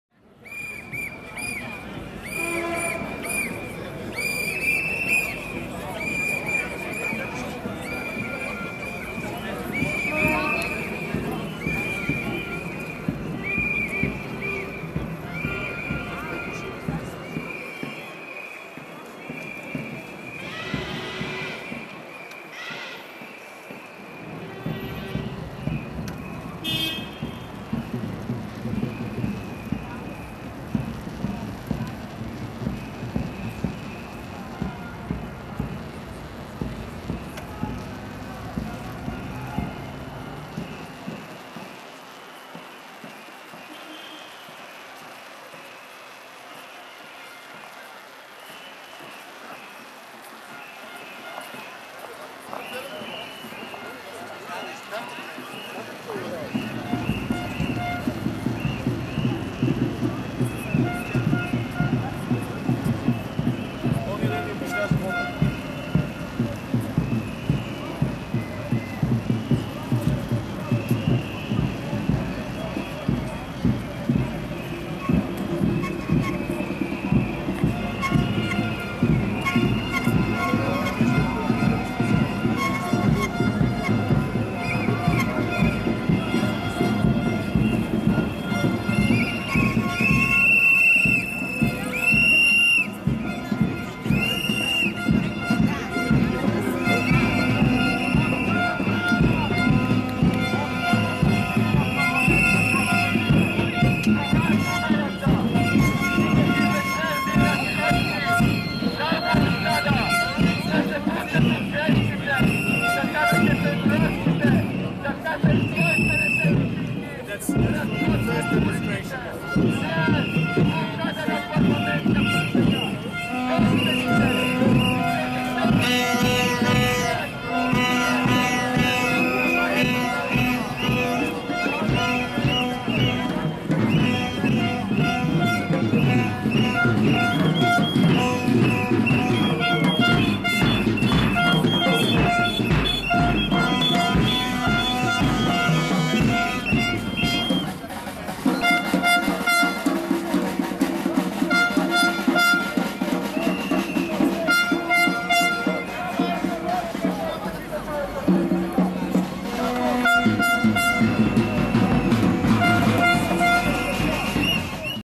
Protest in Sofia 08.11.2013 on Eagles' Bridge in Full HD 3D
Day one hundred forty-eigth of protests in the capital of Bulgaria - Sofia, against the new elected government. Filmed is the crowd from several locations at Eagle's Bridge.

Ден сто четиридесет и осми на протести в столицата на България - София, срещу новоизбраното правителство. Заснета е тълпата от няколко места на Орлов мост.

Видеото е заснето със Сони Хендикем HDR-TD30VE дигитална камера с автоматични настройки на естествена светлина. Звука е автентичен.